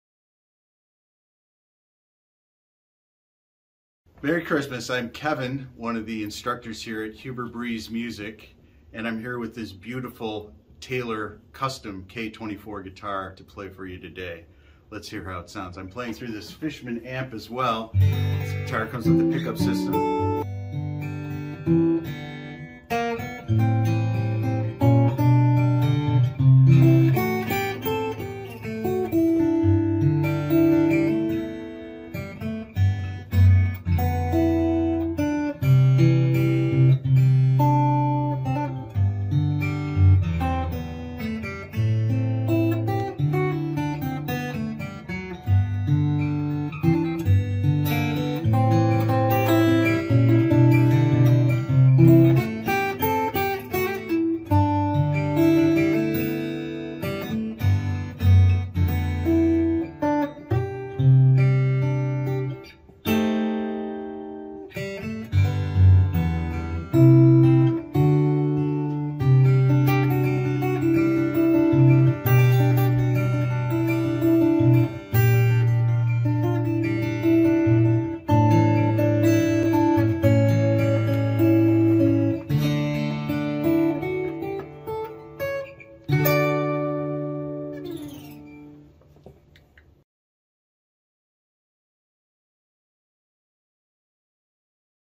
TAYLOR HUBER BREESE 50TH ANNIVERSARY K24CE LTD KOA ACOUSTIC GUITAR
This beautiful Taylor Limited Edition features hand picked KOA, Armrest, and 21:1 Gotoh Tuners.   Check huberbreese.com for detailed description and availability.  Call Robby for best price.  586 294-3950 Mention this video for special price.